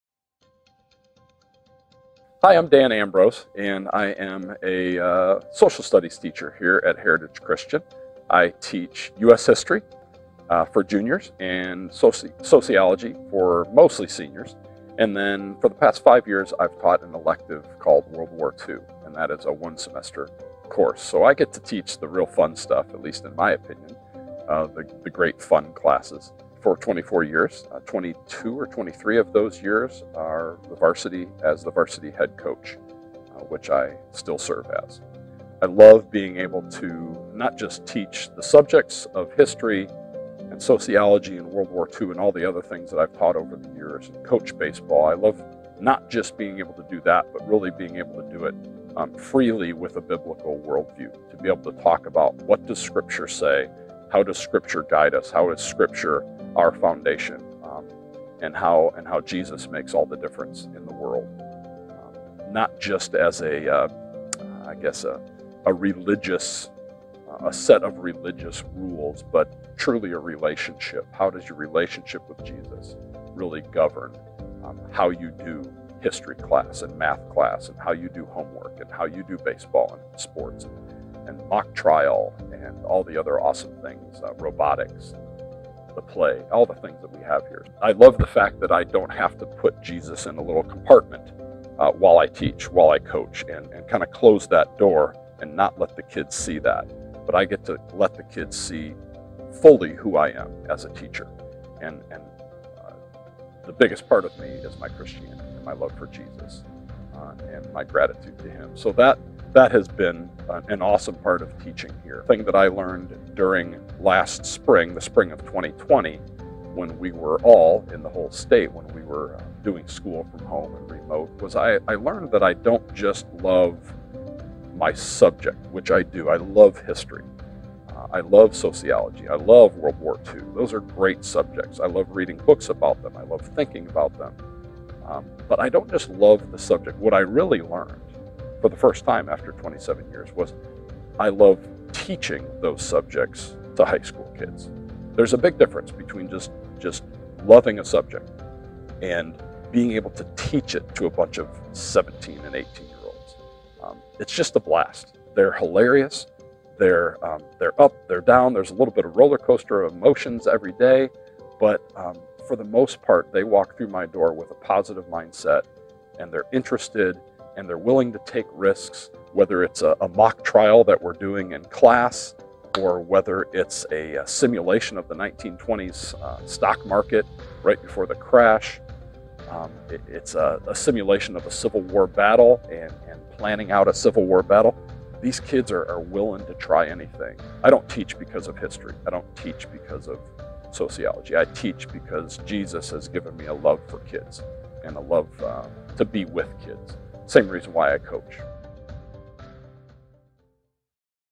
Teacher Feature: Dan Ambrose | Heritage Christian School
High school history teacher Dan Ambrose not only enjoys the subject of history, but he genuinely loves teaching it to high school students.

Heritage Christian School offers Inspiring Education for students in preschool - grade 12 in Indianapolis, IN. Learn more on our website at heritagechristian.net!